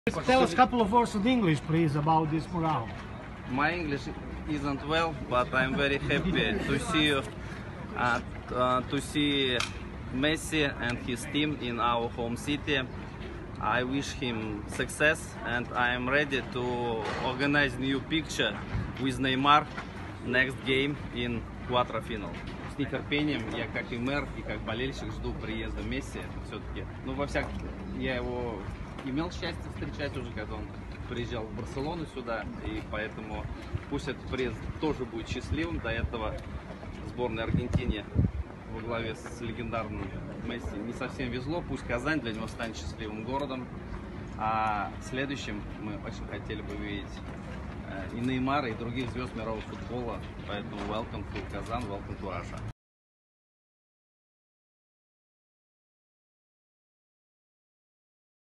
«I'm happy to see Messi»: Метшин ответил иностранному журналисту на вопрос о граффити
Мэр Казани Ильсур Метшин открыл граффити с изображением известного футболиста Лионеля Месси. Рисунок легендарного спортсмена расположился на фасаде здания Казанского регионального центра связи, расположившиеся напротив гостиницы, где остановится аргентинская сборная.
Видео: Владислав Зимагулов
Подготовила: Айгуль Мутыгуллина